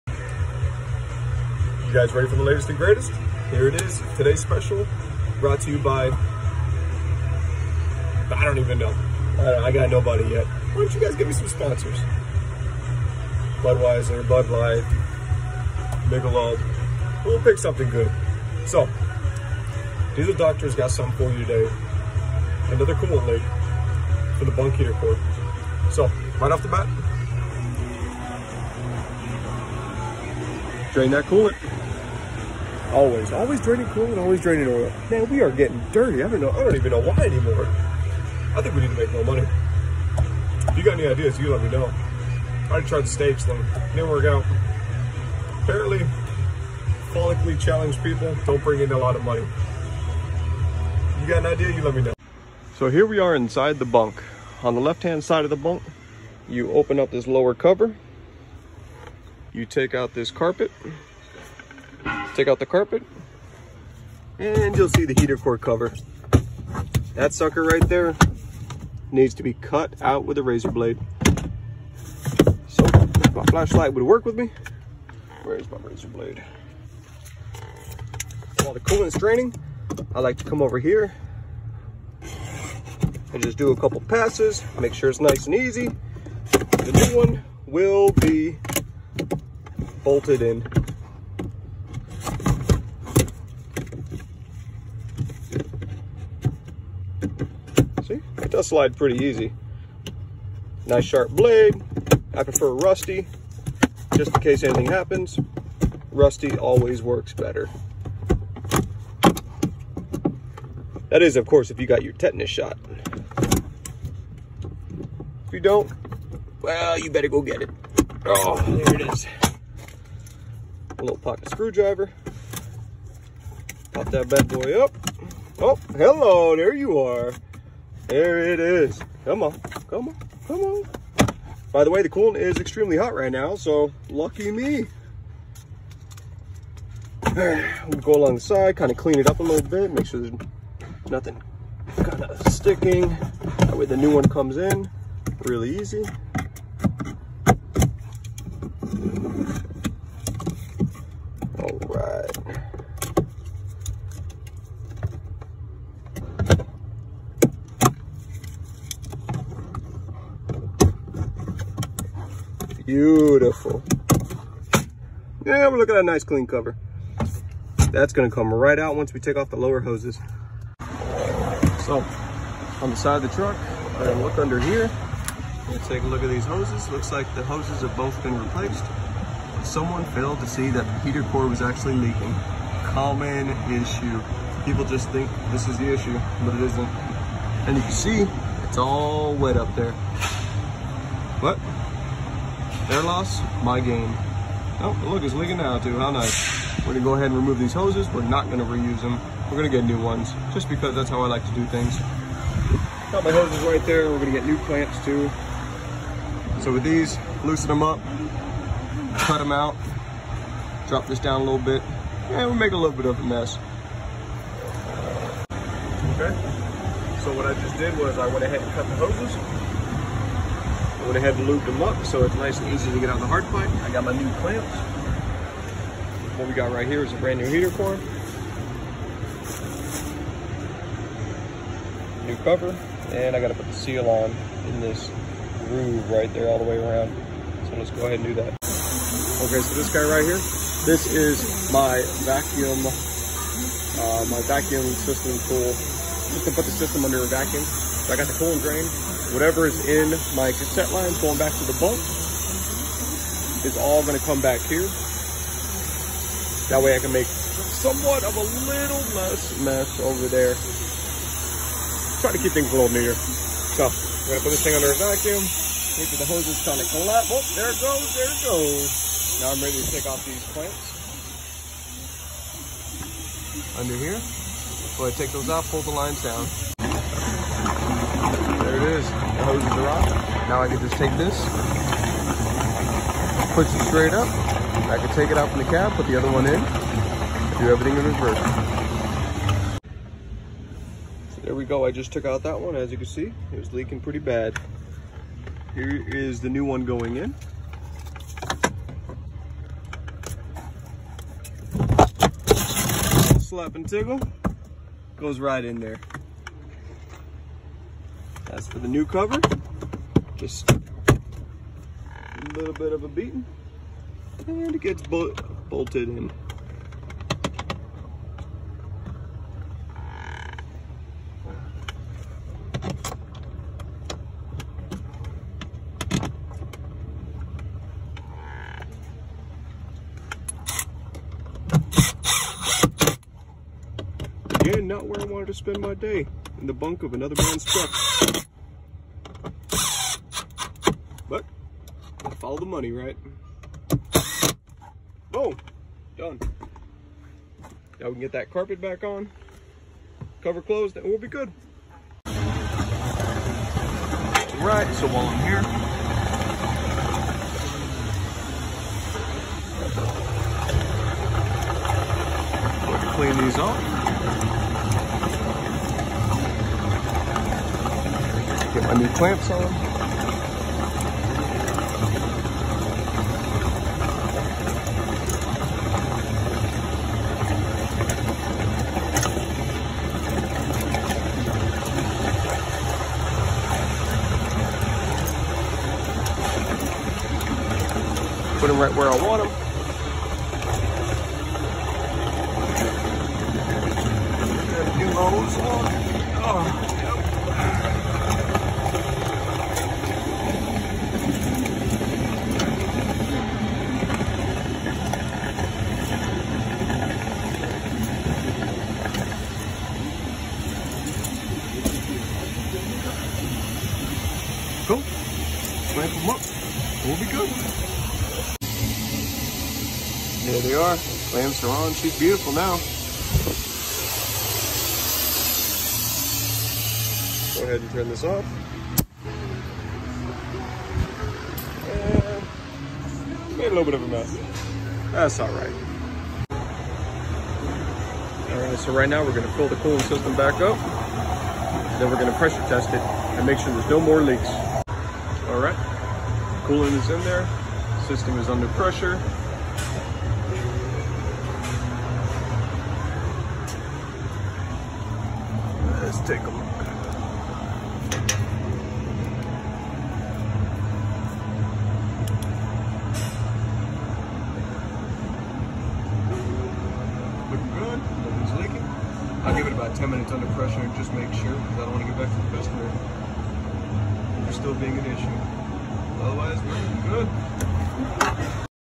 Freightliner cascadia dd15 bunk heater core leaking. Done by diesel dr from busted knuckle
Today I’ll teach you how to avoid bed bugs in a hostile environment. Suit up and prepare for battle inside a driver side cabinet and hope it clean. Watch and see me replace a bunk heater core.

Check me out @ Cookie Monster fitness on insta